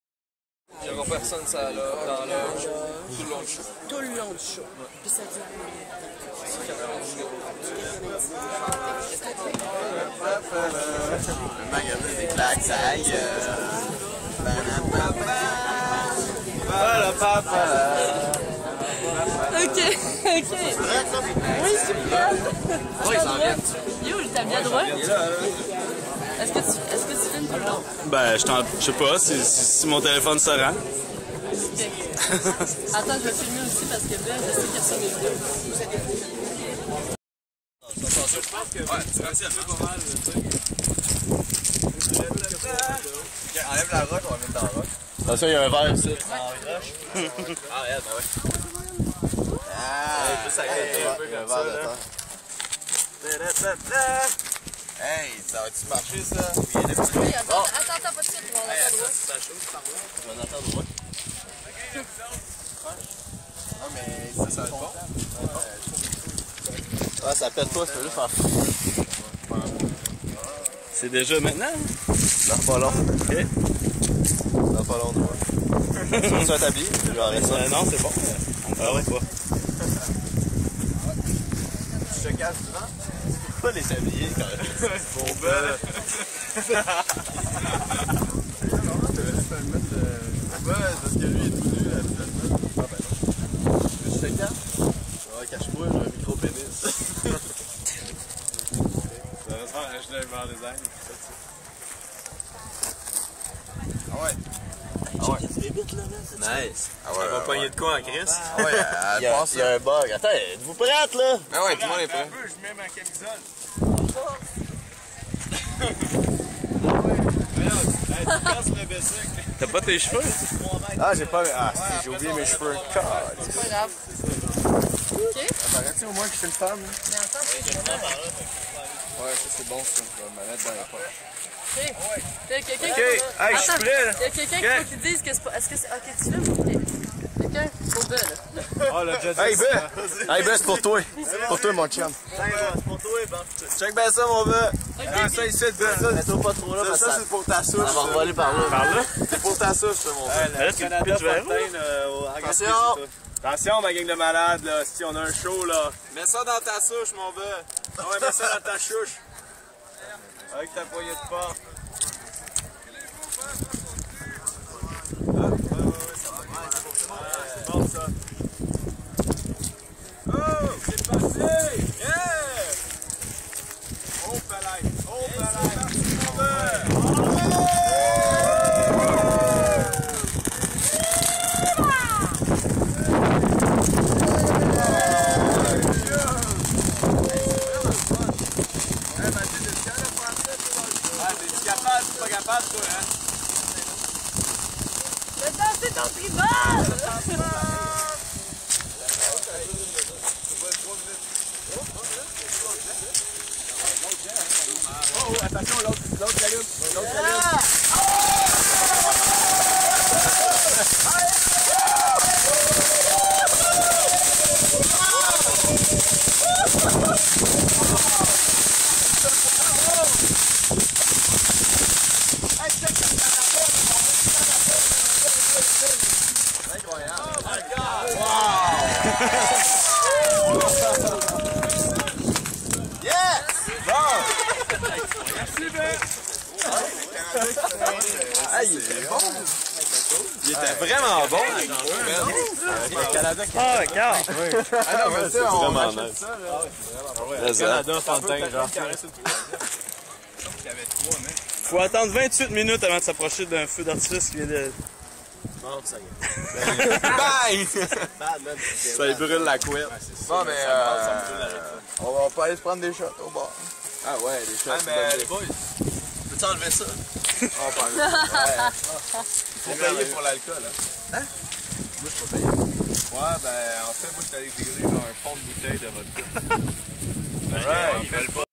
Misteur Valaire et le feu d'artifice du Canada
À Woodstock en Beauce, Benoit a donné un feu d'artifice du Canada à Misteur Valaire. Ils étaient censés le faire péter devant le nouveau couple royal le 1er juillet.

Ils ont été sages au Parlement; ils l'ont gardée (gardéE, parce qu'elle s'appelait Canada "Chantal" Fountain) pour Petite-Vallée, deux jours après.

Pendant l'entrevue (la remise du cadeau) à Woodstock en Beauce, ils portaient des tabliers sexy. Ils ont (presque) recréé l'évènement avec des bavettes à homard sur lesquelles ils avaient dessiné des pectoraux pis toute.

Loïc Thériault à la caméra, le reste de l'équipe à l'émerveillement.

Soyez, vous aussi, émerveillés par la splendeur du Canada.